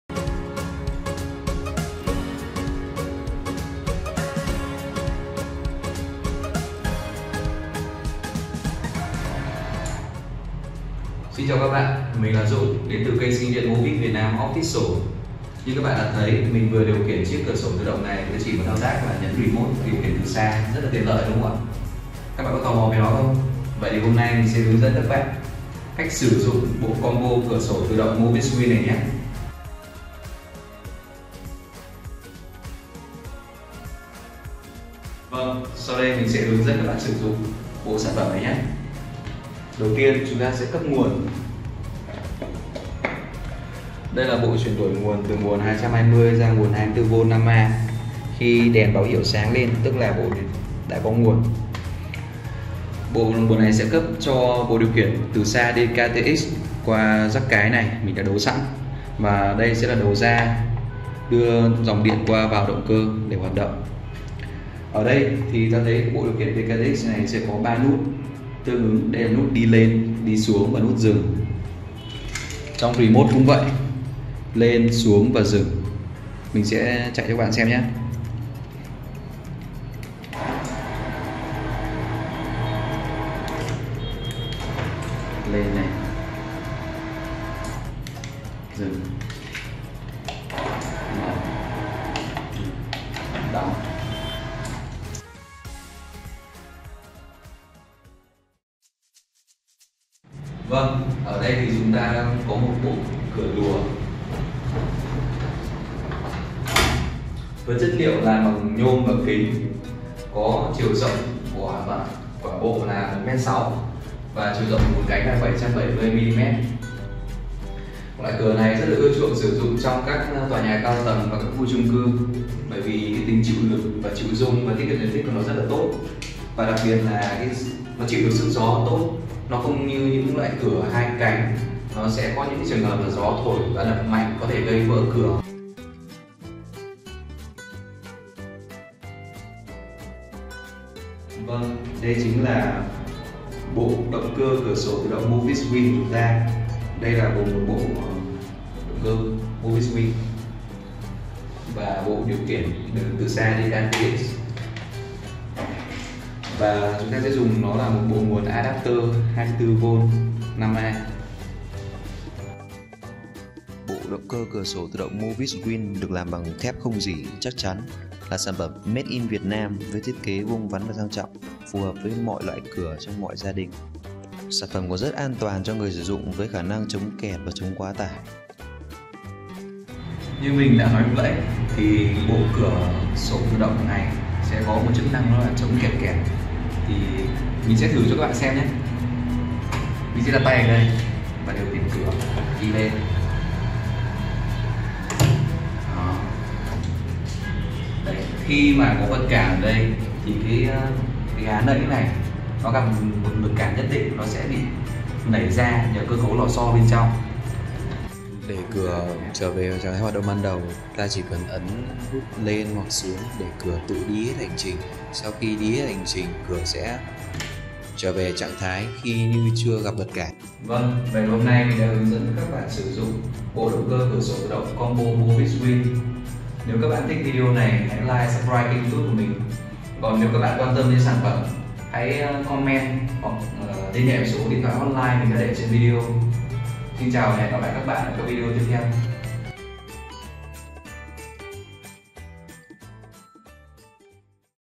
GIỚI THIỆU ĐỘNG CƠ ĐÓNG MỞ CỬA SỔ TỰ ĐỘNG MOVIS WIN
Giới thiệu động cơ đóng mở cửa sổ tự động MOVIS  WIN . Cam kết chất lượng uy tín!
CÔNG TY TNHH thương mại và kỹ thuật MOVIS VIỆT NAM
🏢 Địa chỉ: Tòa H, Chung cư Xuân Mai Complex Dương Nội, P. Yên Nghĩa, Q. Hà Đông, TP. Hà Nội